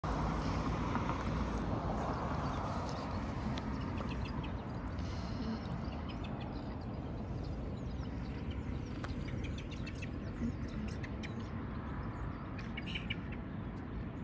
Patitos
Aunque son pequeños y frágiles, los patitos derrochan valentía y determinación. Aprenden a confiar en sus instintos y se sumergen en el agua con destreza, buscando alimento y aventuras. Su espíritu intrépido es una inspiración para todos aquellos que los observan, recordándonos que la valentía y la curiosidad son ingredientes esenciales para vivir una vida plena.